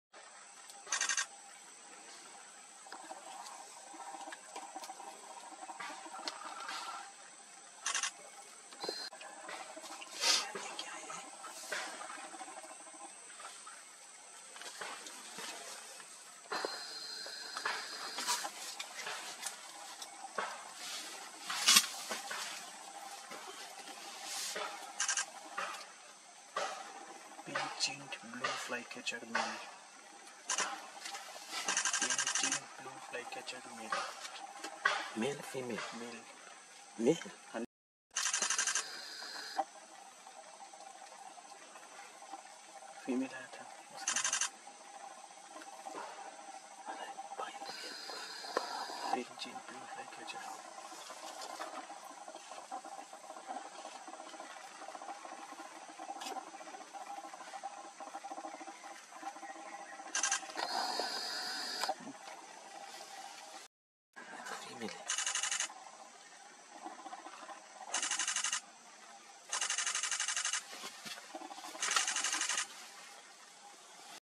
Pale-chinned Blue Flycatcher - 2 | Bird | Wildlife | Forest | Dosdewa Forest Trail | Karimganj
Pale-chinned Blue Flycatcher - 2 | Bird | Wildlife | Forest | Dosdewa Forest Trail | Karimganj, is a video taken by Shrikant Kelkar at Dosdewa Forest, Karimganj, Assam, India in October 2022

Binomial Name : Cyornis poliogenys ; Family : Muscicapidae